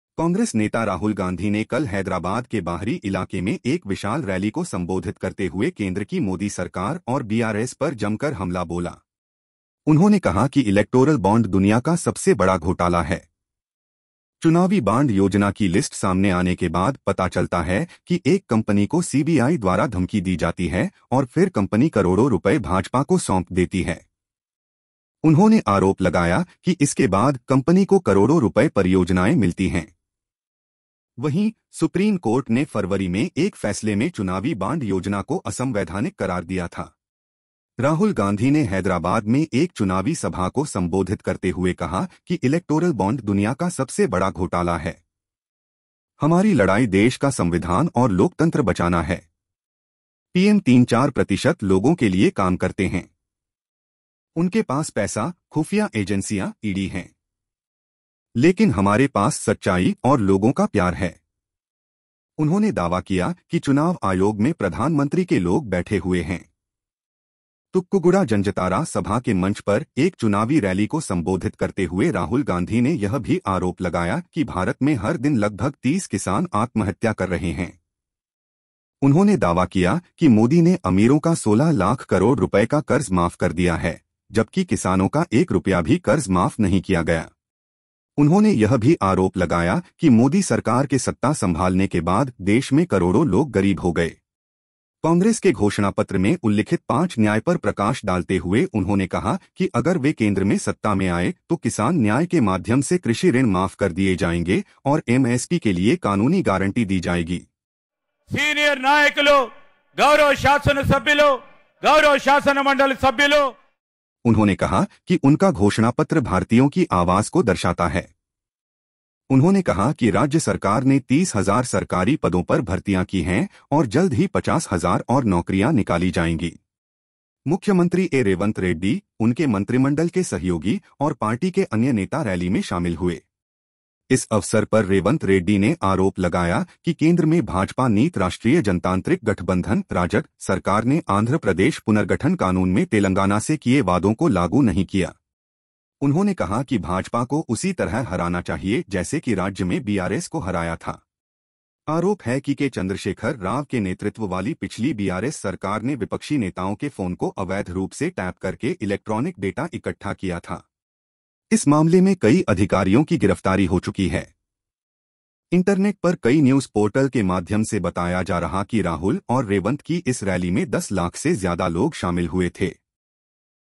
तेलंगाना के हैदराबाद से राहुल गाँधी ने विशाल जनसभा को संबोधित किया | Congress Manifesto Launch Rally
Former Congress President Mr. Rahul Gandhi launches the Congress manifesto and addresses the public in Hyderabad, Telangana.

कांग्रेस नेता राहुल गांधी ने कल हैदराबाद के बाहरी इलाके में एक विशाल रैली को संबोधित करते हुए केंद्र की मोदी सरकार और बीआरएस पर जमकर हमला बोला. देखिये पूरा विडियो !